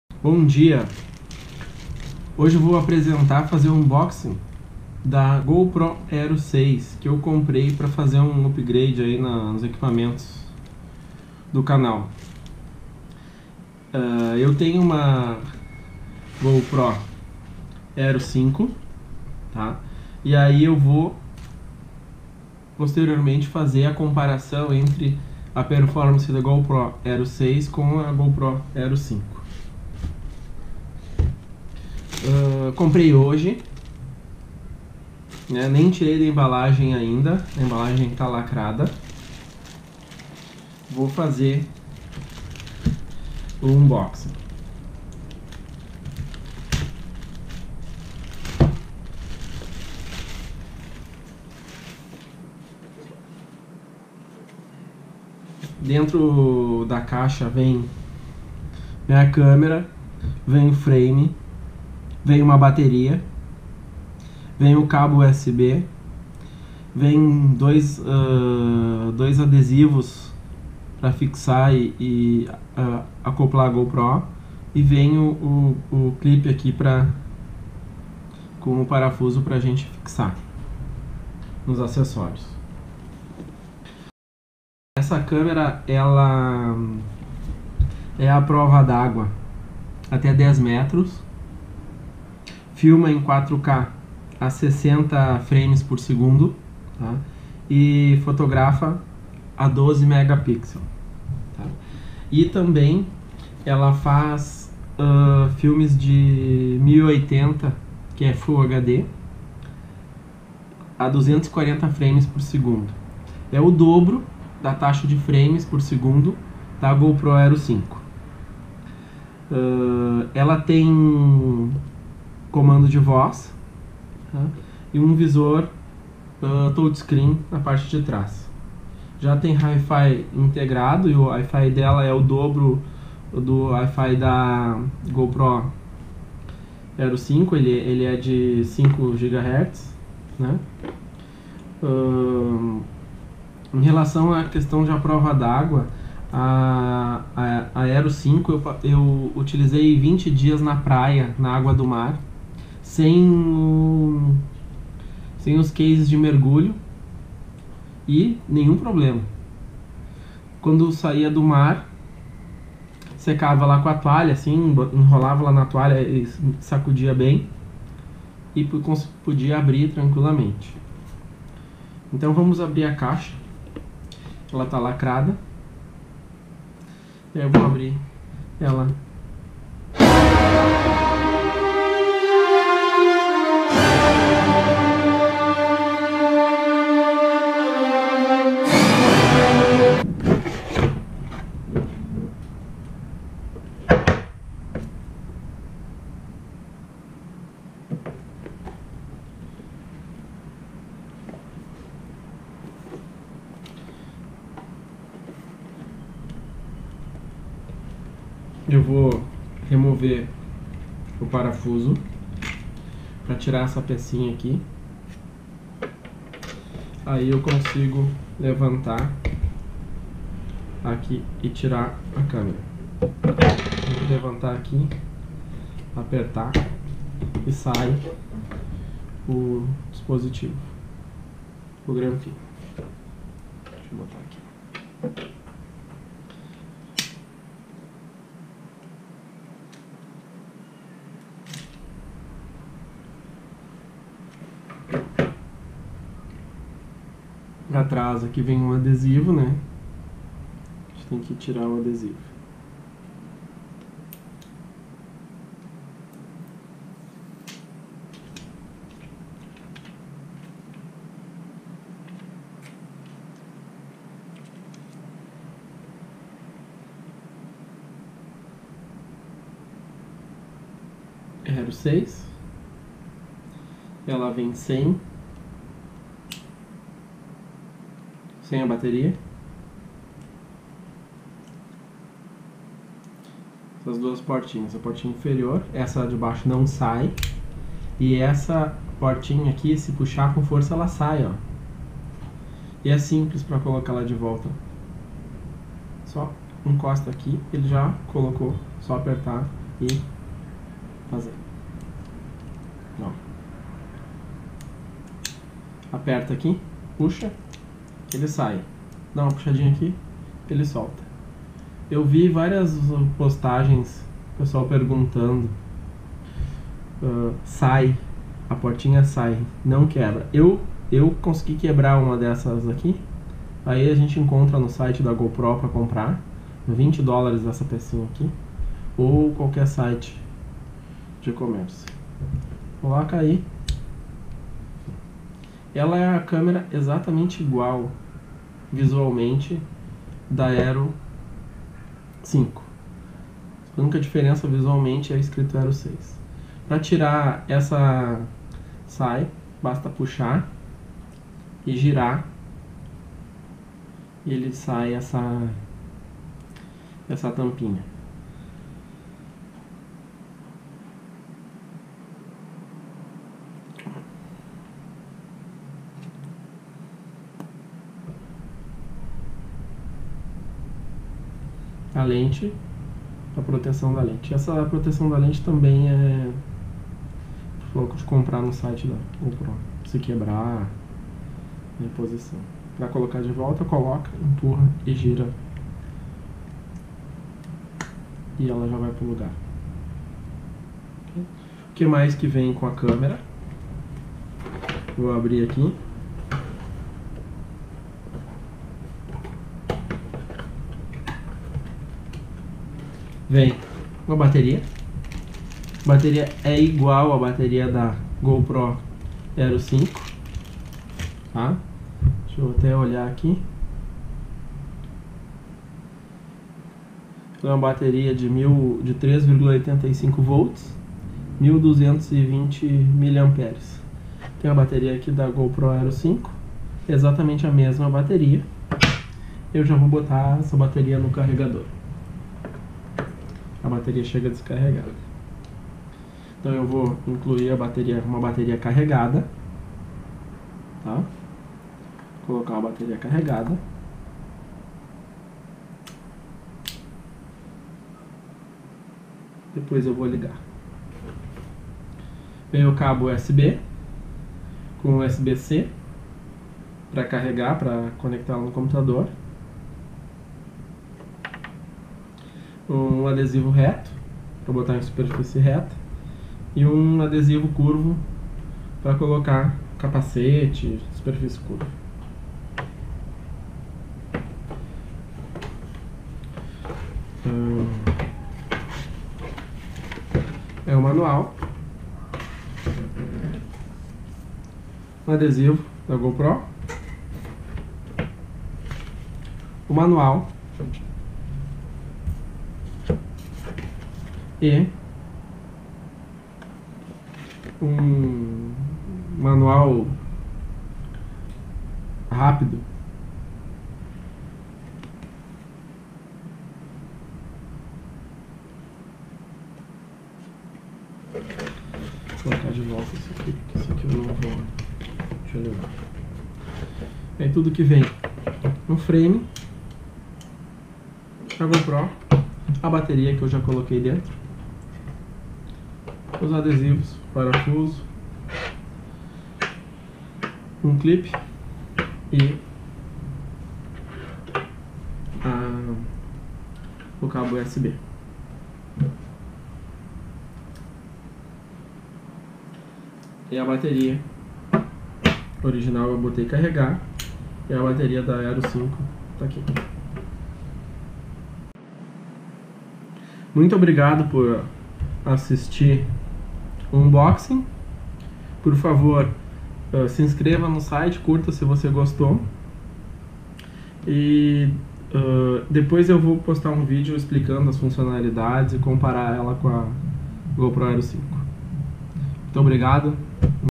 Unboxing Gopro Hero 6
Abertura da caixa da gopro hero 6.

Para tirar as dúvidas da galera!

Mostro como colocar a porta lateral e a proteção da lente.